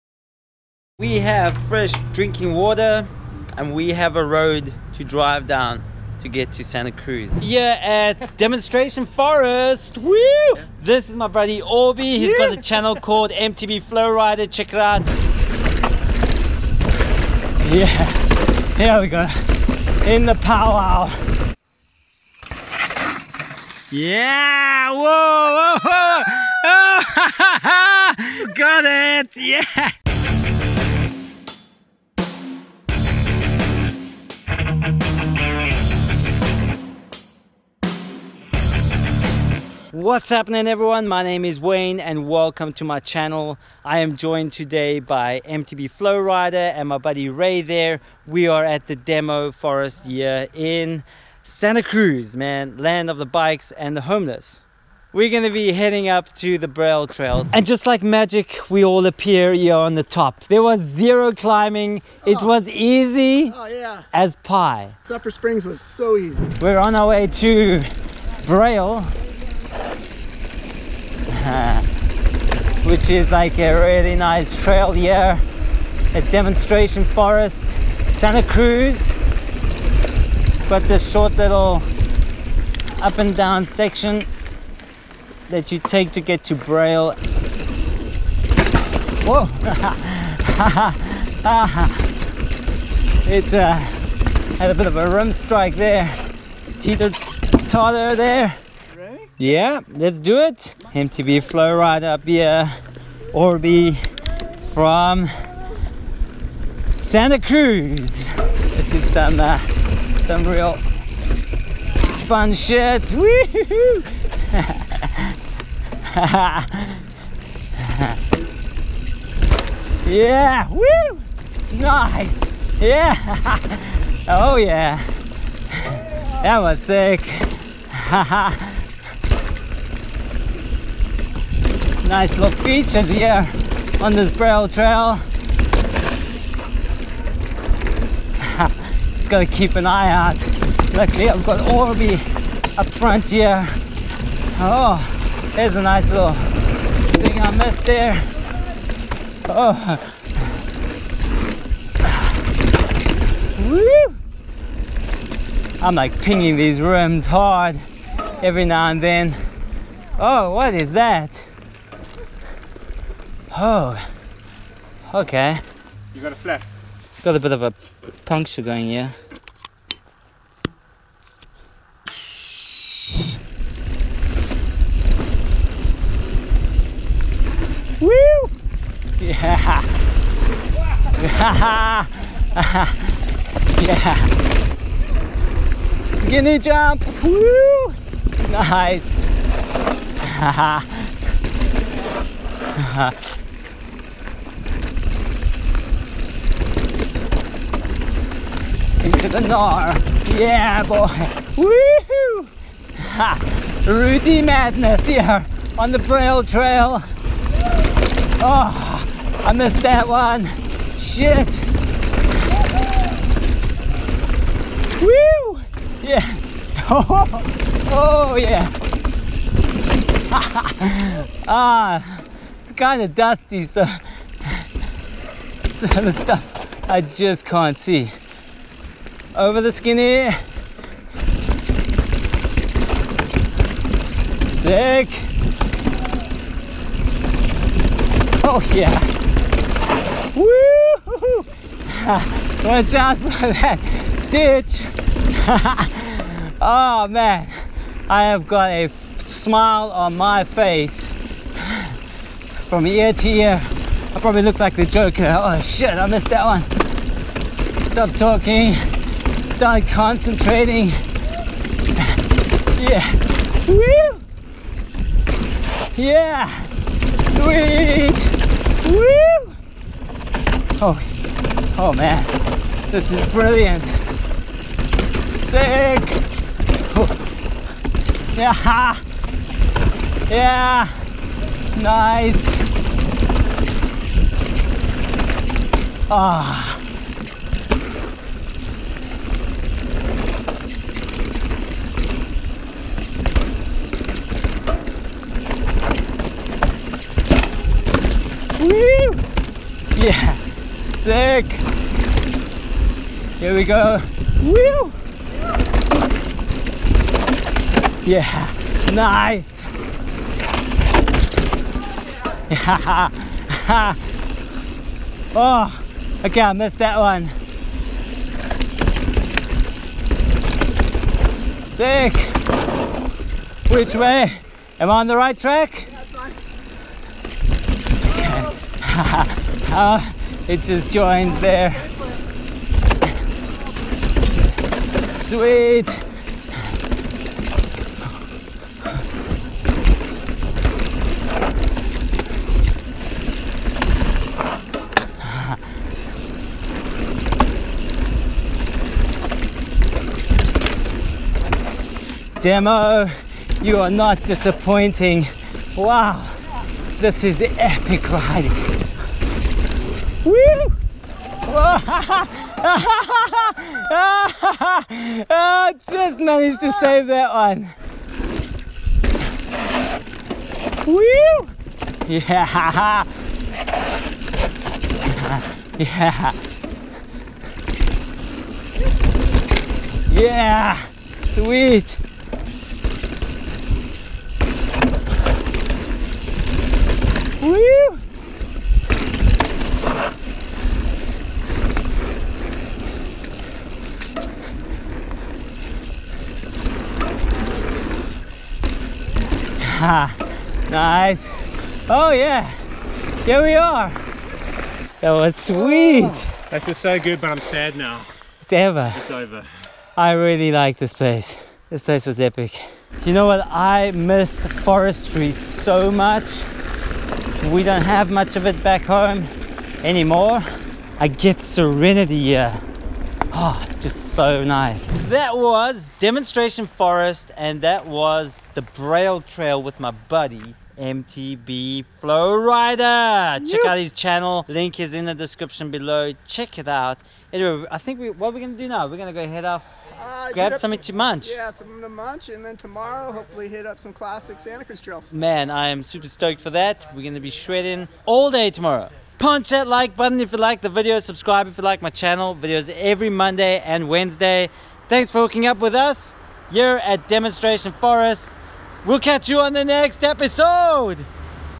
Black Diamond SANTA CRUZ DH Trail | Mountain Bike Trails Northern California
We are being shown around again today by MTB Flowrider who has vast knowledge of his home trails in Santa Cruz.

The Braille Trail is a black rated trail located in Demonstration Forest in Santa Cruz. It's just over 2km of fun & excitement and will have you smiling from ear to ear while on there.

The day we were  out there the temperatures were average, peaking at around 30 Deg Celsius but you are riding under some of the tallest trees I have ever seen and the canopy protects you pretty well from the sun.

The trail conditions were extremely dusty and some areas had blown out corners but that just makes the ride more fun.